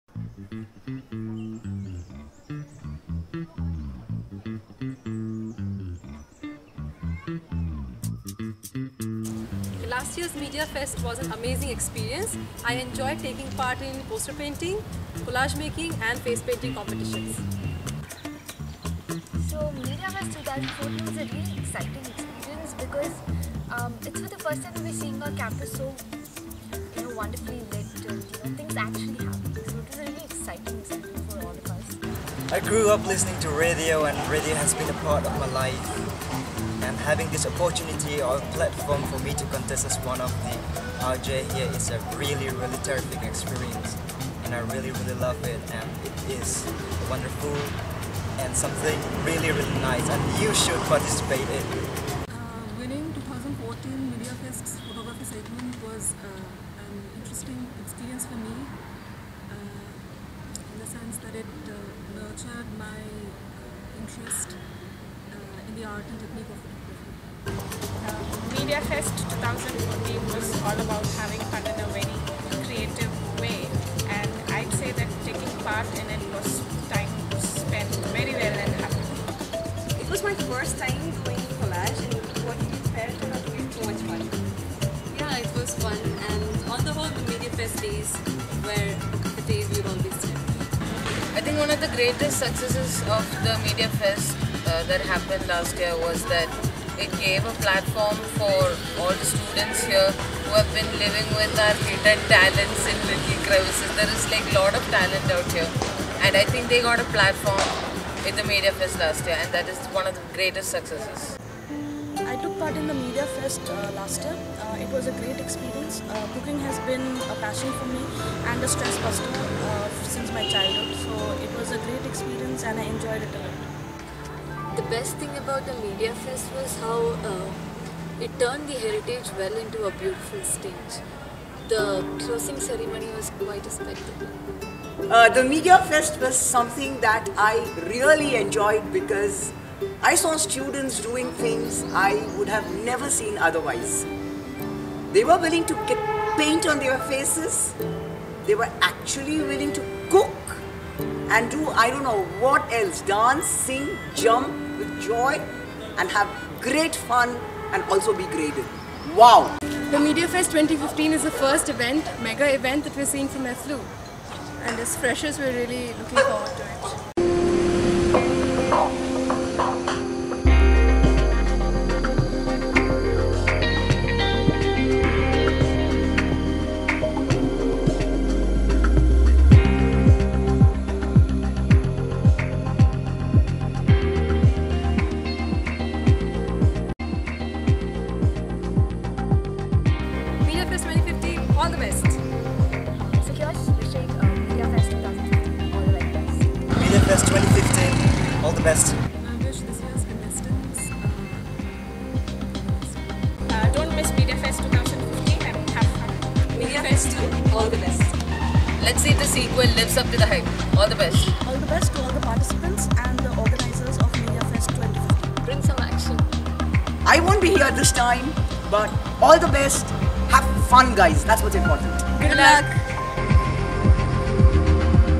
Media Fest 2015 (Promo)
Promo video of Media Fest 2015 being organised by the Department of Communication, The English and Foreign Languages University (Hyderabad).